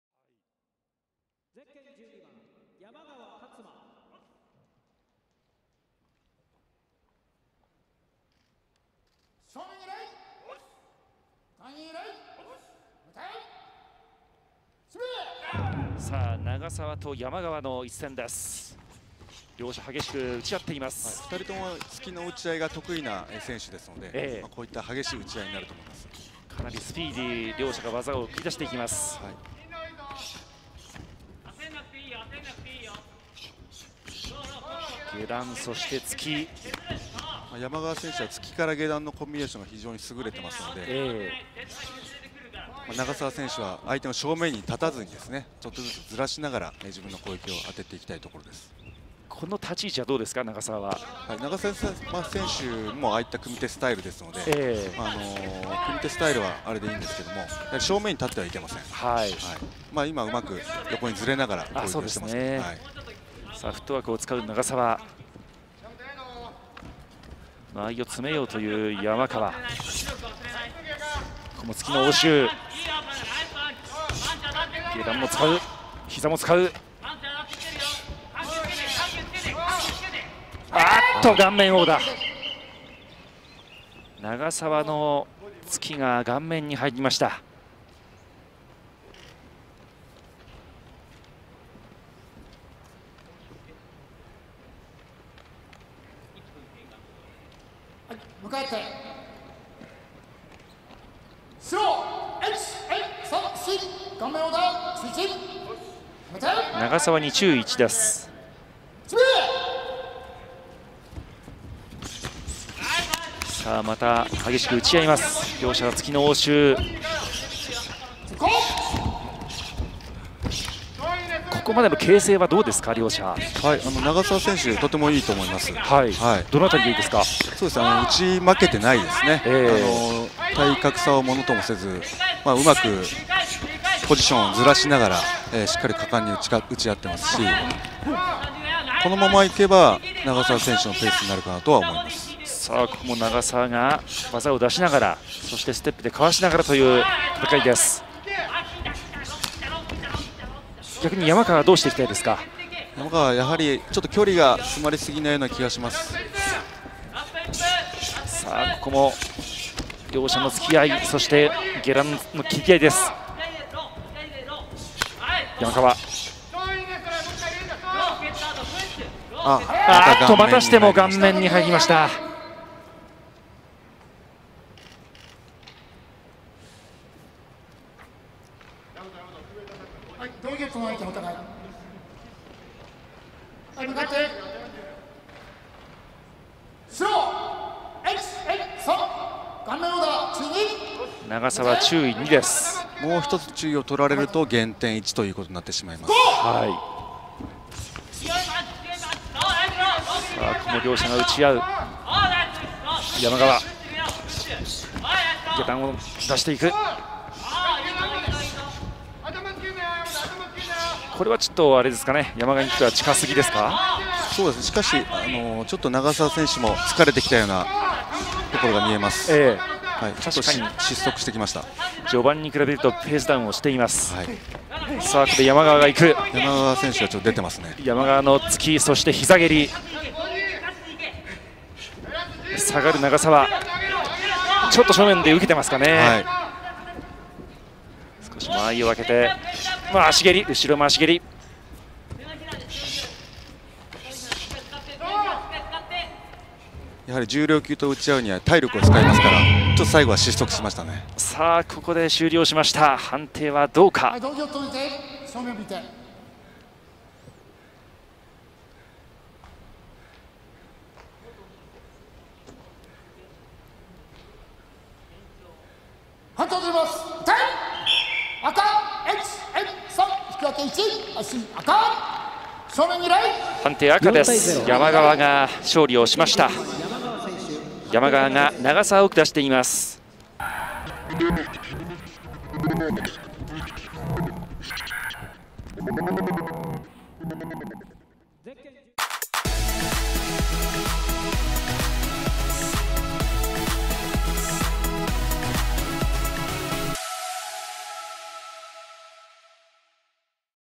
山川竜馬 VS 長澤大和【Ryoma Yamakawa VS Yamato Nagasawa】
2020年 第52回全日本空手選手権 3回戦【2020 52nd All Japan Karate Championship Round 3】

世界各地で行われている空手の試合動画です。
This is a video of karate games being held all over the world.